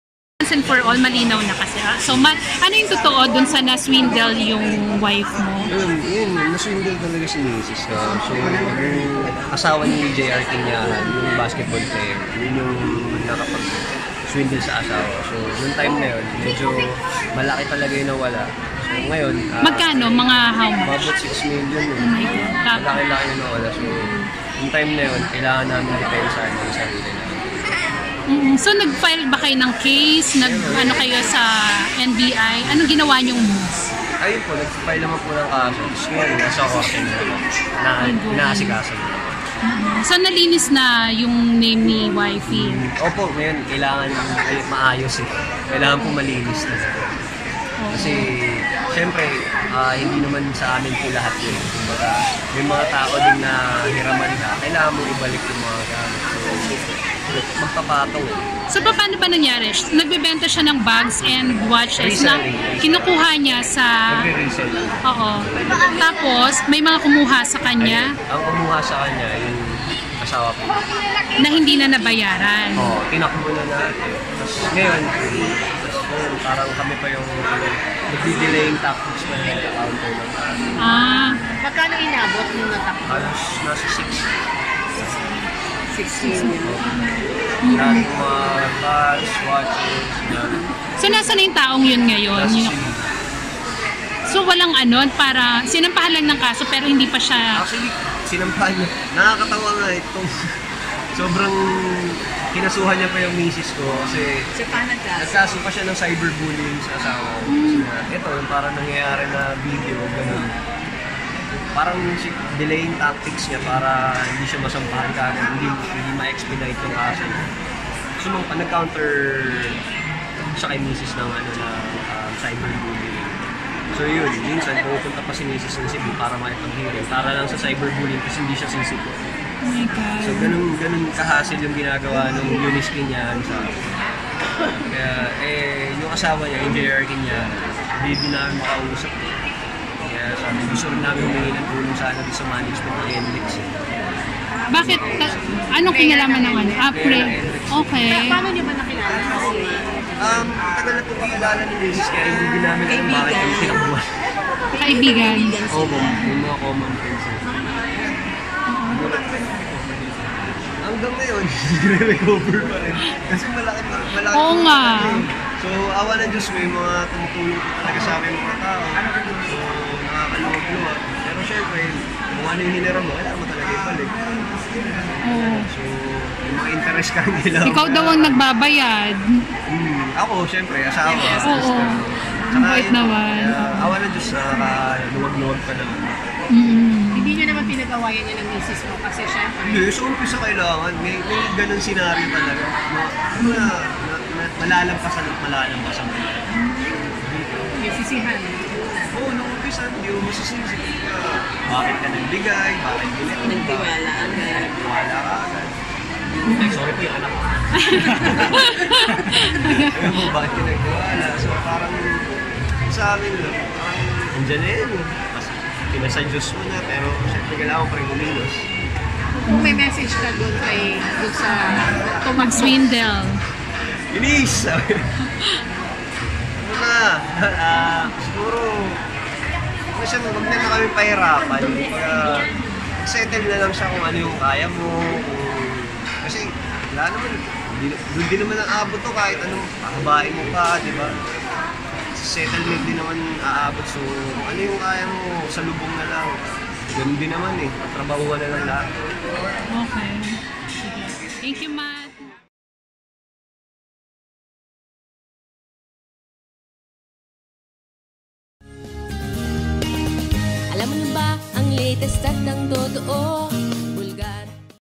Misis ng basketbolista, wanted sa panloloko ng P6 Milyon kay Matt at sa asawa
Misis ng basketbolista, wanted sa panloloko ng P6 Milyon kay Matt at sa asawa.. Chika pa more by Ateng Janiz